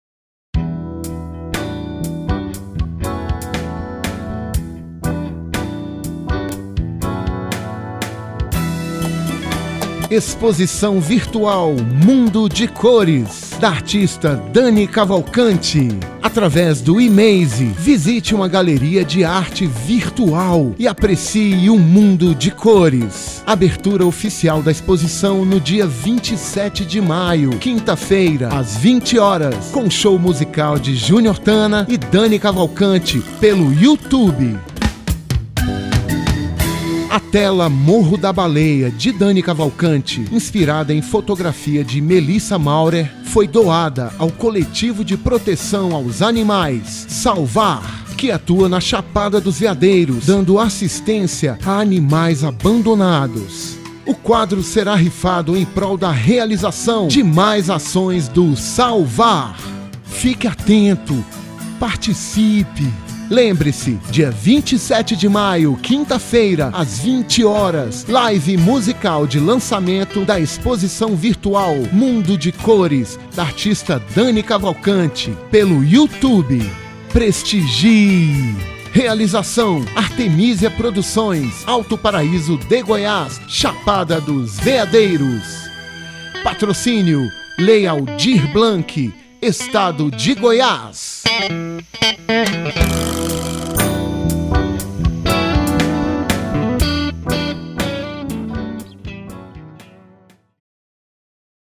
TEASER - Exposição Virtual 'MUNDO DE CORES' de Dani Cavalcante - Lei Aldir Blanc n°14.017/2020
Exposição Virtual ‘MUNDO DE CORES’ de Dani Cavalcante.

Através da plataforma EMAZE, visite uma Galeria de Arte Virtual
e aprecie um mundo de cores! (O link de acesso à exposição no EMAZE
será divulgado no dia da Live)

Abertura Oficial da Exposição no dia 27 de maio, quinta–feira, às 20h,
com o show musical de Júnior Tana e Dani Cavalcante. pelo FACEBOOK,
na página de Dani Cavalcante.

A tela ‘MORRO DA BALEIA’, de Dani Cavalcante, foi doada ao Coletivo de
Proteção aos animais SALVAR, que atua na Chapada dos Veadeiros,
para ser rifado ou vendido, e ajudar na realização de suas ações.
Fique atento! Participe!

Dia 27 de Maio, quinta-feira, às 20h, LIVE MUSICAL de lançamento da
Exposição Virtual ‘MUNDO DE CORES’, da artista Dani Cavalcante.

PRESTIGIE!

Realização ARTEMÍSIA Produções – Alto Paraíso de Goiás –
Chapada dos Veadeiros - Preserve a Natureza

Realização com recursos da Lei Aldir Blanc, do Governo Federal, Ministério do Turismo, Secretaria Especial de Cultura, Governo de Goiás e Secretaria de Estado de Cultura de Goiás.